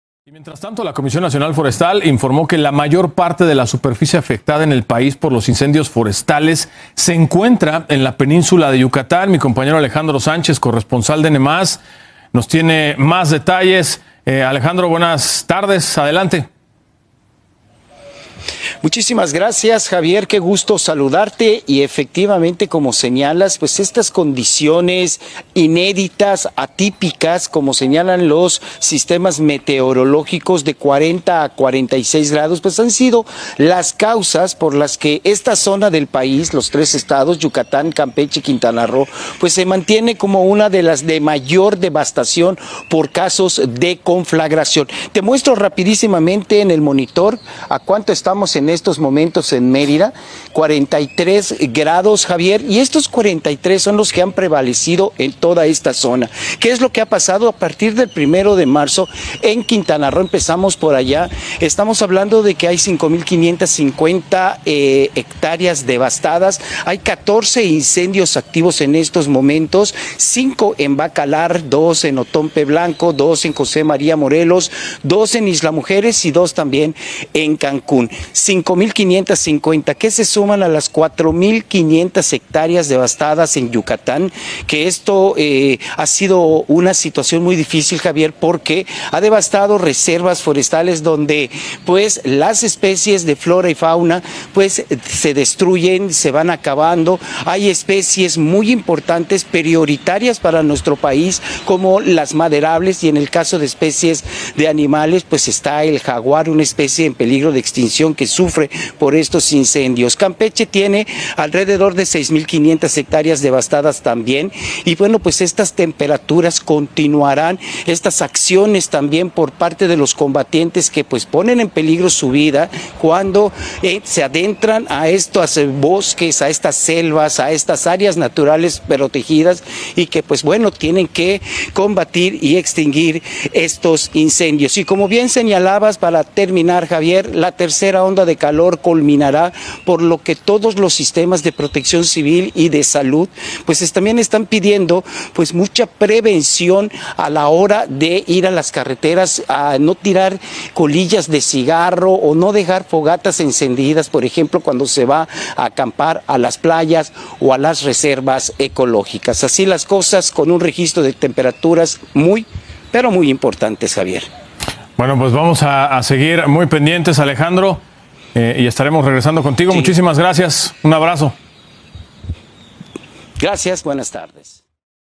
INCENDIO FORESTAL EN YUCATAN
Incendios Devastan Más de 16,500 Hectáreas en la Península de Yucatán
La Comisión Nacional Forestal reportó que la mayor parte de la superficie afectada por los incendios en México se encuentra en la Península de Yucatán